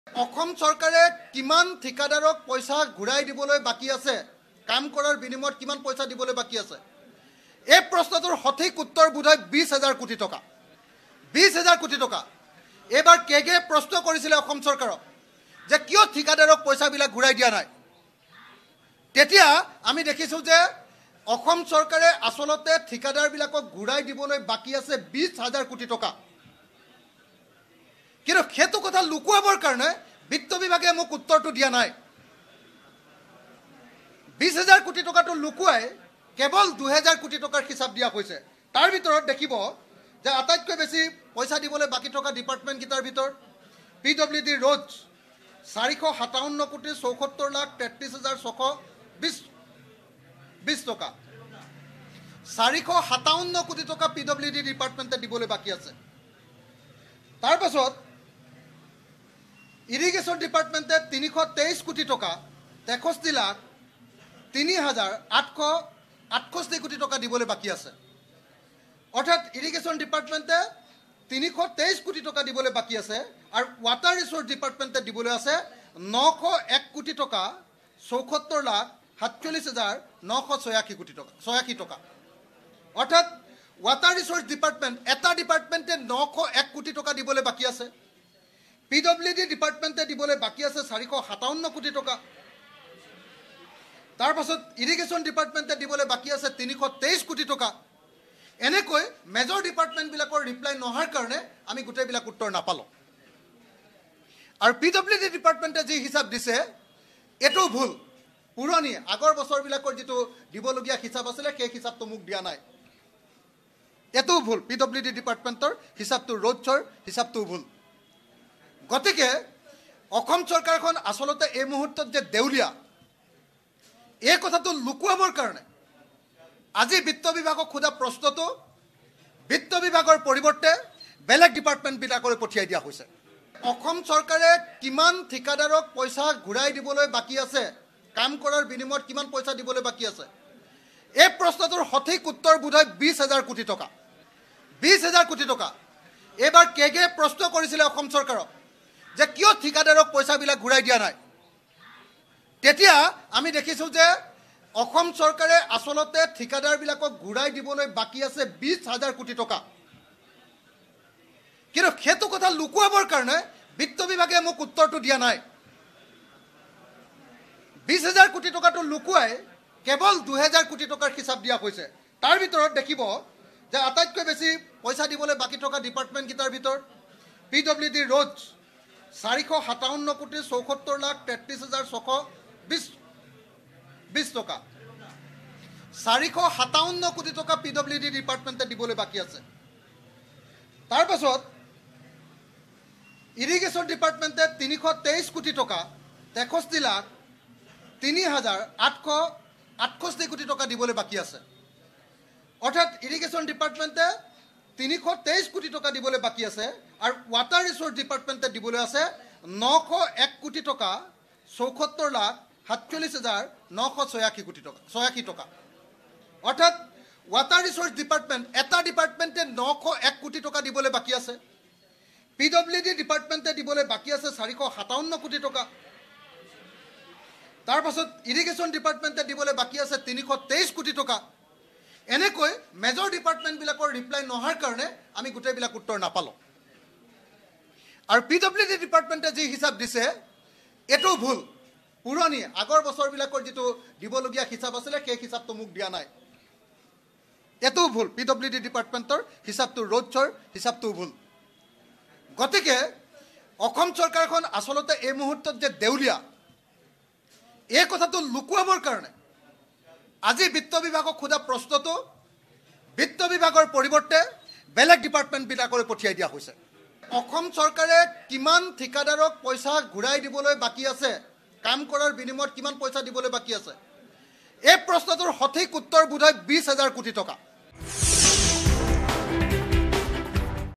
'অসম চৰকাৰে ঠিকাদাৰসকলক দিবলৈ বাকী আছে ২০হাজাৰ কোটি টকা'।
'অসম চৰকাৰে ঠিকাদাৰসকলক দিবলৈ বাকী আছে ২০হাজাৰ কোটি টকা'। বিত্ত বিভাগে বিধানসভাৰ মজিয়াত তথ্য লুকুওৱাৰ অভিযোগ অখিল গগৈৰ।